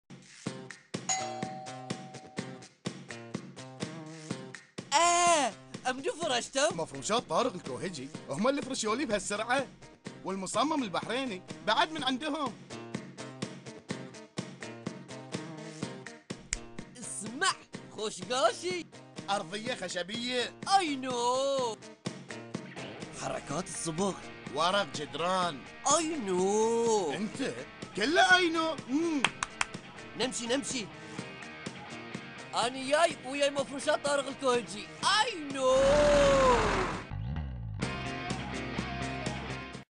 دعاية مفروشات طارق الكوهجي (آي نوووو)
دعاية مفروشات طارق الكوهجي - رمضان 2014
تمثيل: علي الغرير ، خليل الرميثي